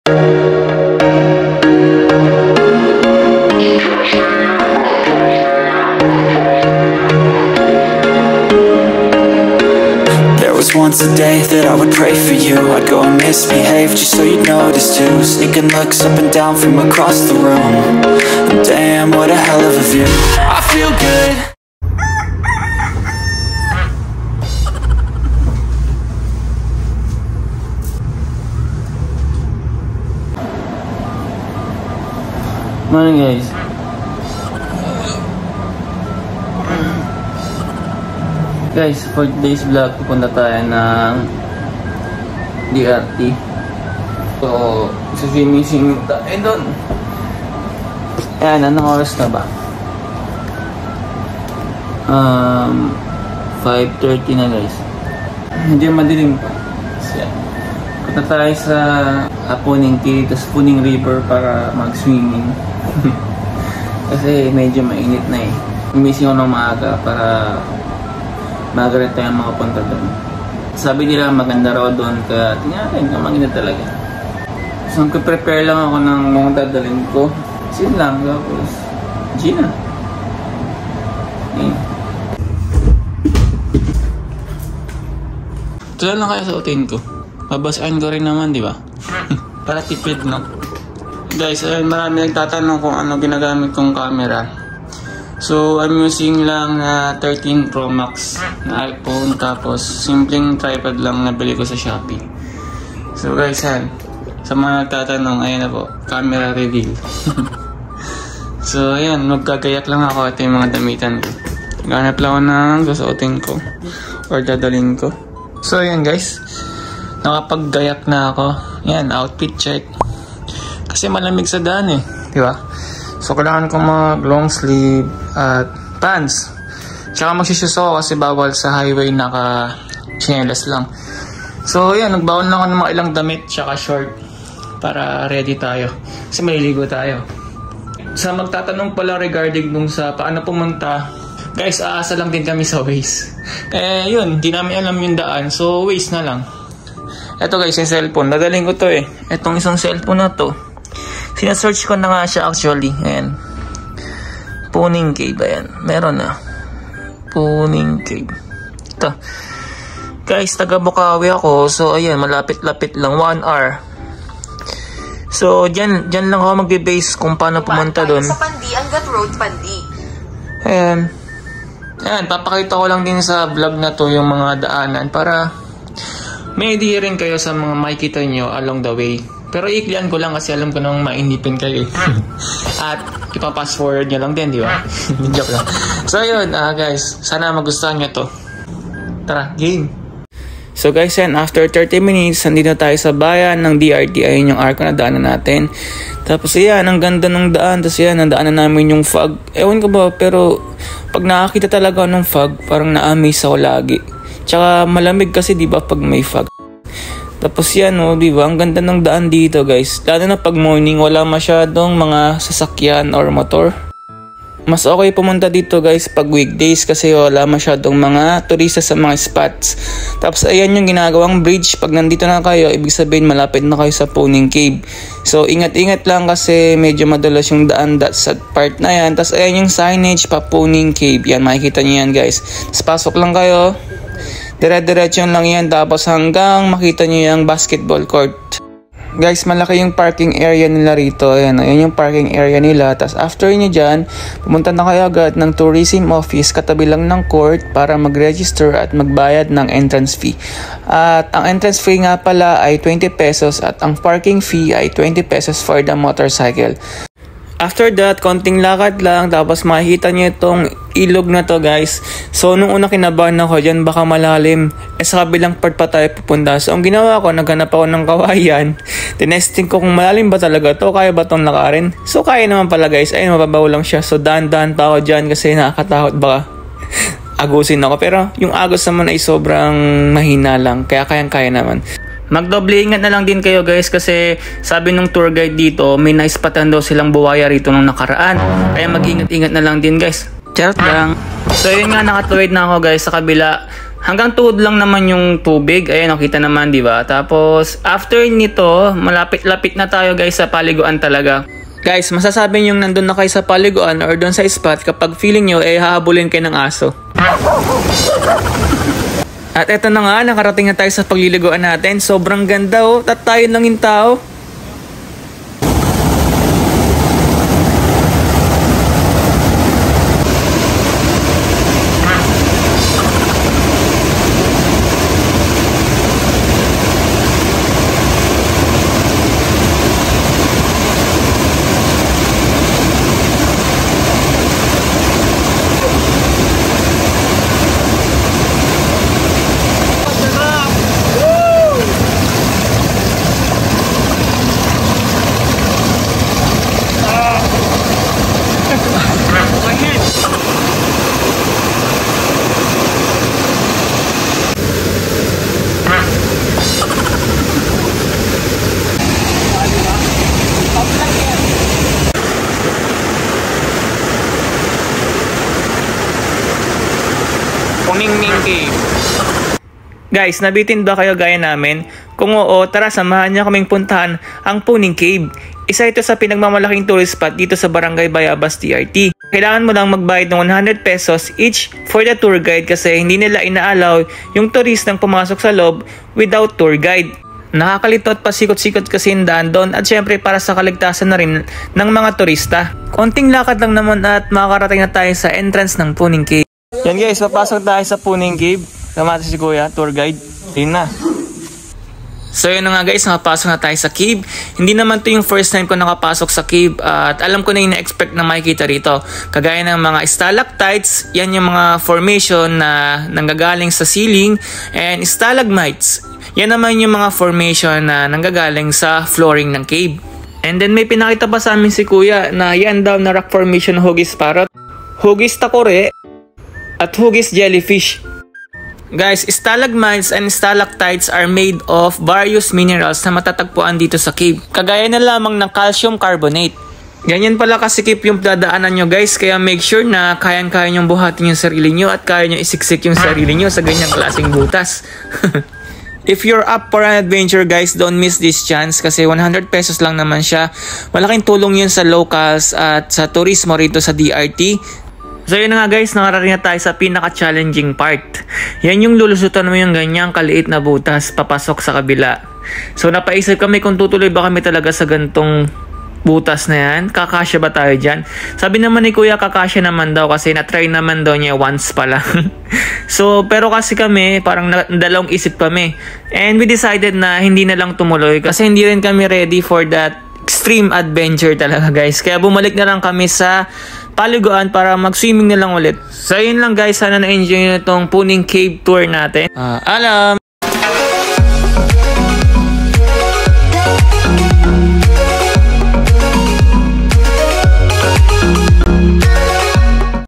Puning Cave | ₱20 Entrance Fee | Mark Mendoza
Hello, i’m Mark Mendoza. This is my 6th vlog, featuring a budget-friendly tourist spot saaming lugar sa Bulacan. Tara, samahan niyo kaming puntahan ang Puning Cave, River & Mini falls. Isa ito sa madami pang maipagmamalaking pasyalan sa DRT, Bulacan ⛰️🌴

Entrance Fee: 20 pesos lang, unli swimming kana 😊🫶🏻

How to get there: JUST USE WAZE APP FOR THE DIRECTION. YUN LANG DIN GINAMIT NAMIN AT HINDI NAMAN KAMI NALIGAW 😊

ENJOY WATCHING! And i hope, may matutunan kayo after 😊

Let’s connect!